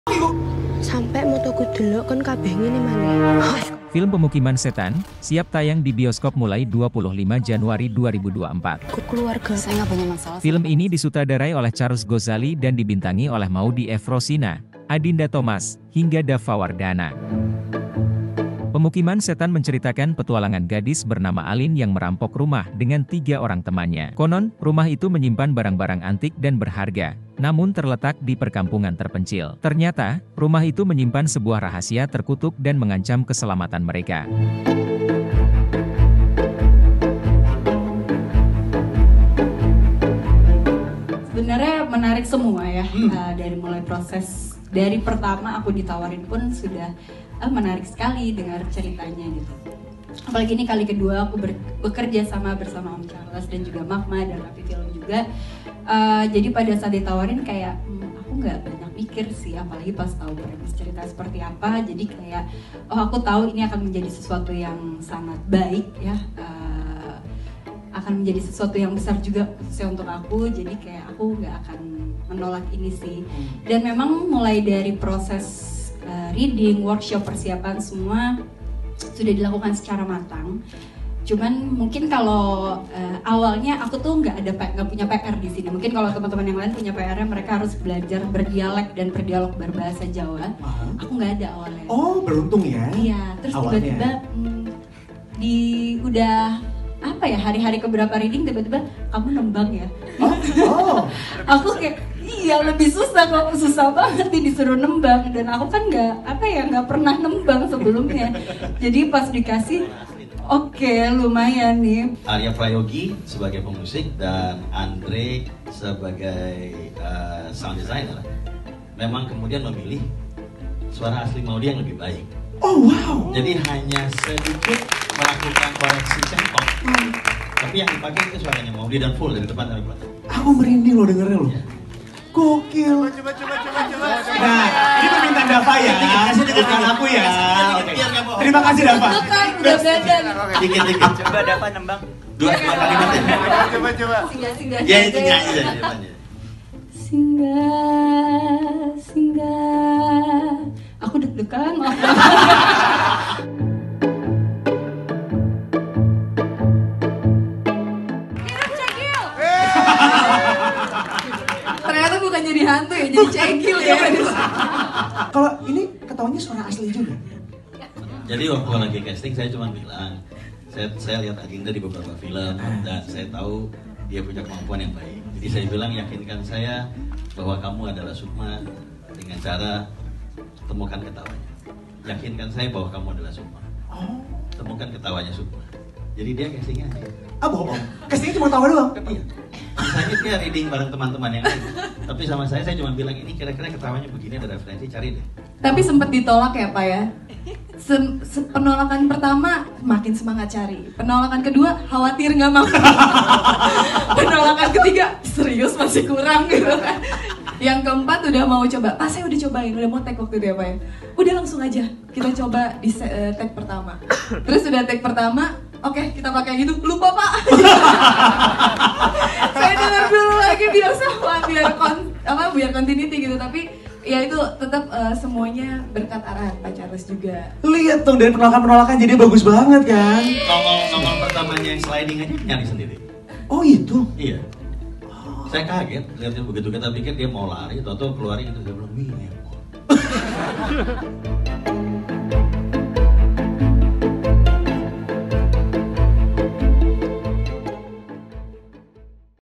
Maudy Effrosina Dan Adinda Thomas Harus Bisa Nembang & Ketawa Serem Demi 'PEMUKIMAN SETAN'
KLovers,
Adinda Thomas membintangi sebuah film horor berjudul PEMUKIMAN SETAN yang disutradarai oleh Charles Gozali. Menariknya, kali ini ia memerankan karakter wanita menyeramkan bernama Sukma. Begitu dinyatakan lolos casting, Adinda diminta mencari bentuk tawa khas yang aneh untuk menghidupkan karakter Sukma. Butuh percobaan beberapa kali sebelum Charles merasa cocok. Selain tawa yang aneh, Adinda Thomas membuat gerakan tubuh khusus serta belajar bahasa Jawa untuk menghidupkan peran Sukma. Setelah syuting, ia terbawa logat Jawa selama beberapa hari.

SINOPSIS:
Alin (Maudy Effrosina), seorang perempuan korban trauma kekerasan keluarga dan himpitan ekonomi mengikuti tiga temannya dan merampok sebuah rumah yang konon menyimpan banyak barang-barang antik yang berharga di perkampungan terpencil. Tanpa mereka sadari, di rumah itu ada sebuah rahasia terkutuk yang akan mengancam keselamatan jiwa mereka semua.